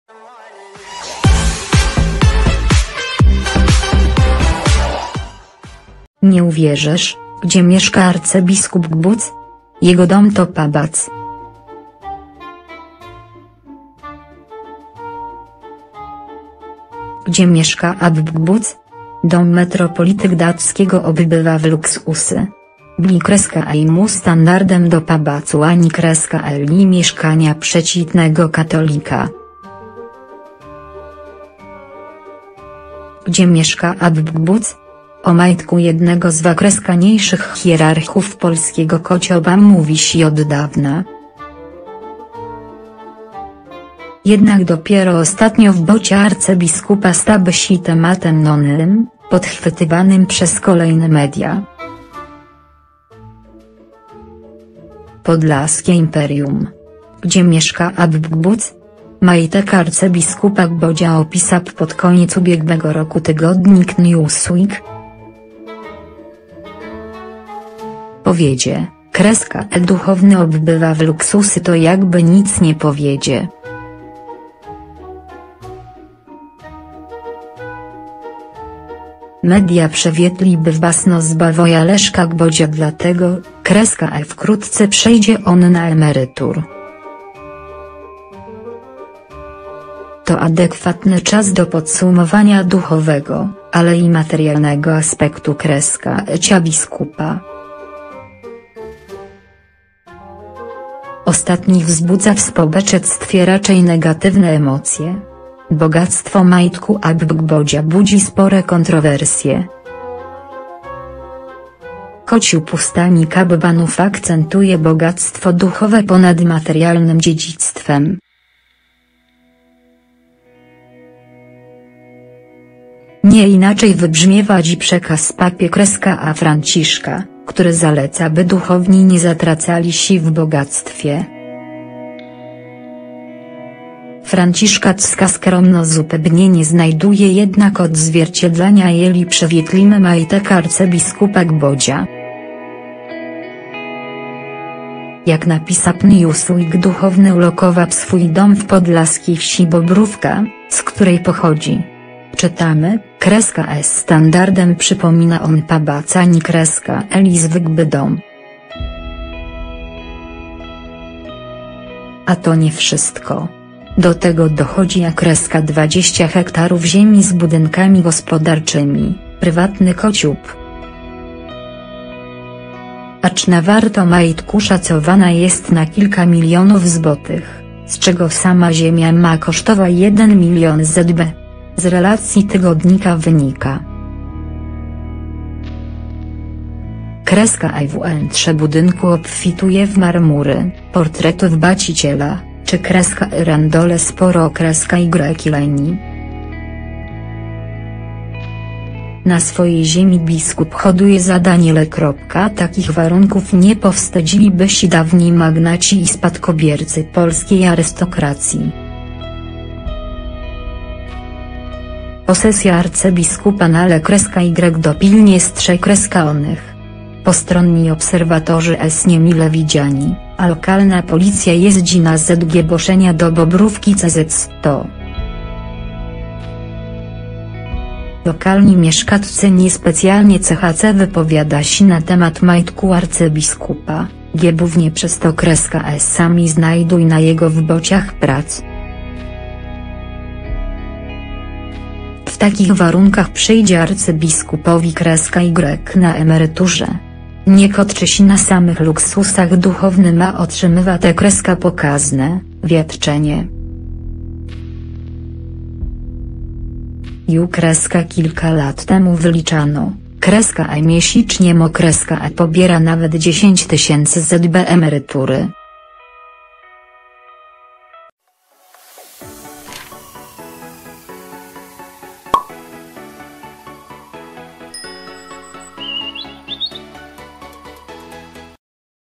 Nie uwierzysz, gdzie mieszka arcybiskup Głódź! Jego dom to pałac - Aktualności 24/7
Nie uwierzysz, gdzie mieszka arcybiskup Głódź! Jego dom to pałac